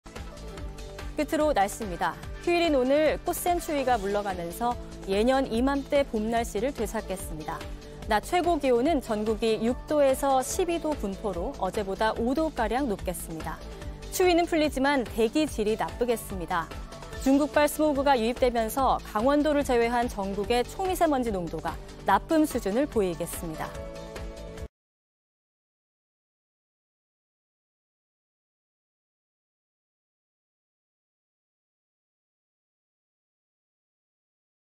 [날씨] 꽃샘추위 물러가자 미세먼지 기승 / JTBC News
휴일인 오늘(3일)은 꽃샘추위가 물러가면서 예년 이맘때 봄 날씨를 되찾겠습니다.

낮 최고기온은 전국이 6도에서 12도의 분포로 어제보다 5도가량 높겠습니다.

추위는 풀리지만 대기 질이 나쁘겠습니다.

중국발 스모그가 유입되면서 강원도를 제외한 전국의 초미세먼지농도가 '나쁨' 수준을 보이겠습니다.